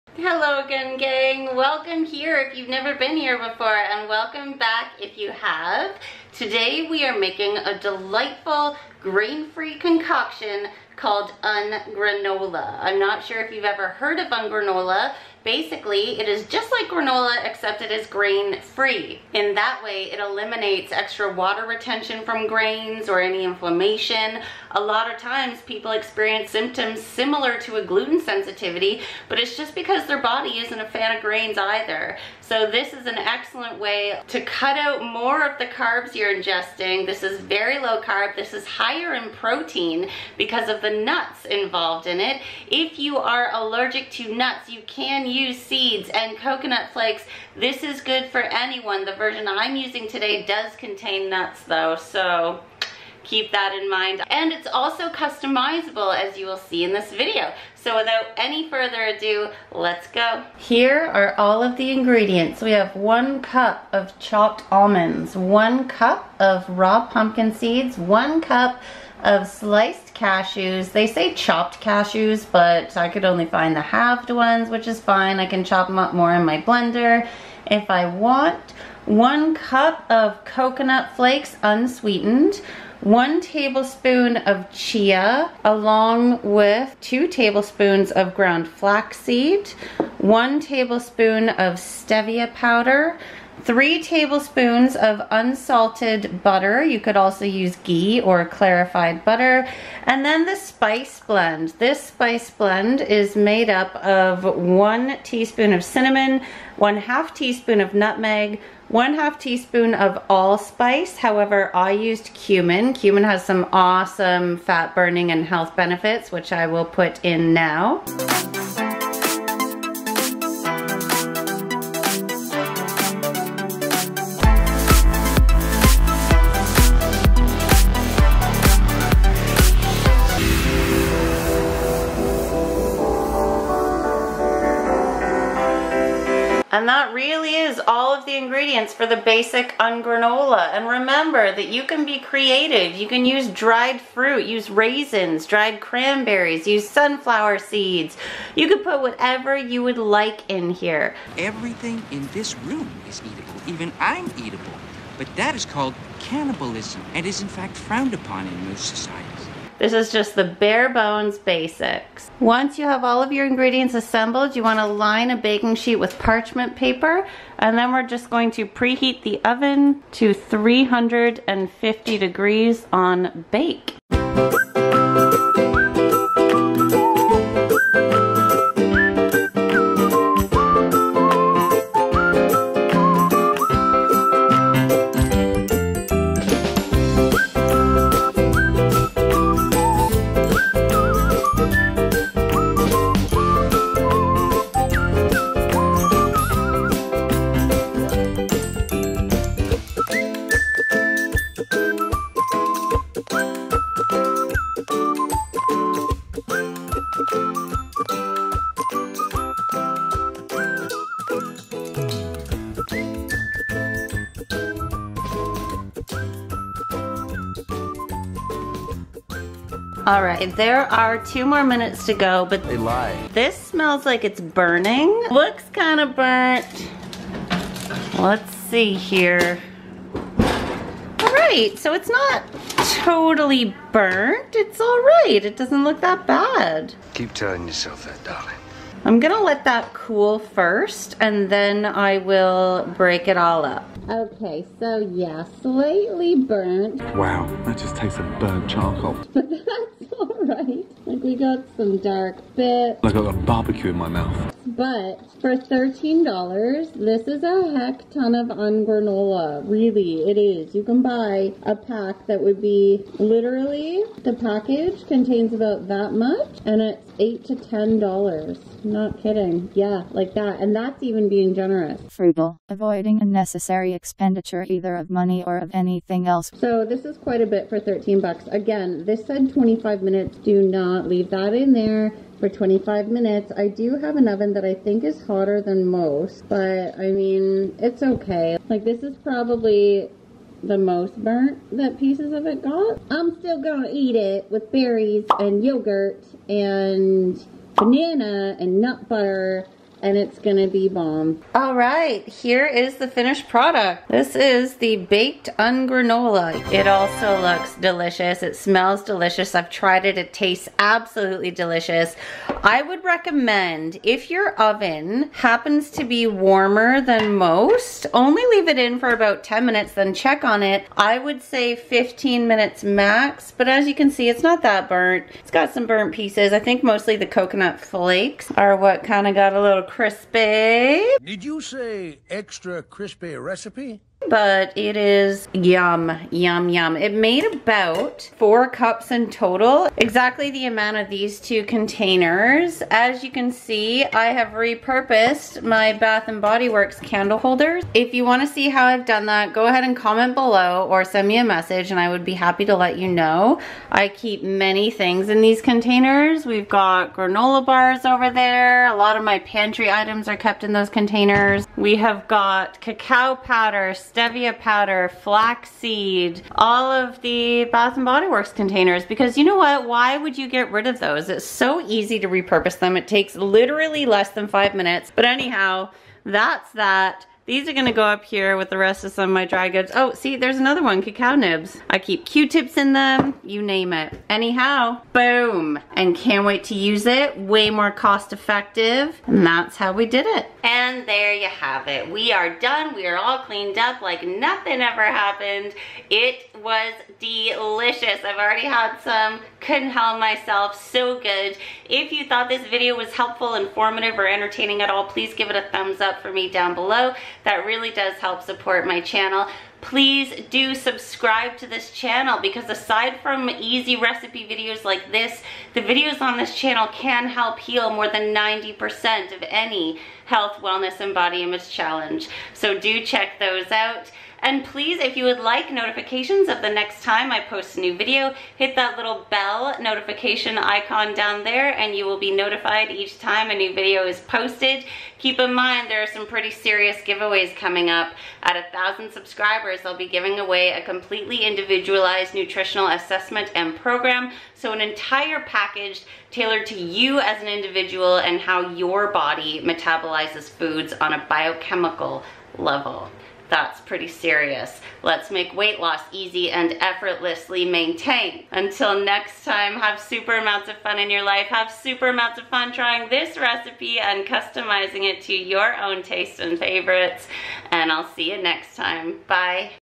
Grain Free Granola Recipe! Anti-inflammatory Un-Granola
Un-granola has been taking the world by storm, and it costs a TON in the store. I've figured out how to EASILY make this grain free granola option at home, for far cheaper! Check out this delicious and less expensive, easy recipe to add to your food favourites today!

Kelly Duffin
Advanced Holistic Health Practitioner
Individualized Nutrition Specialist
Personal Training Specialist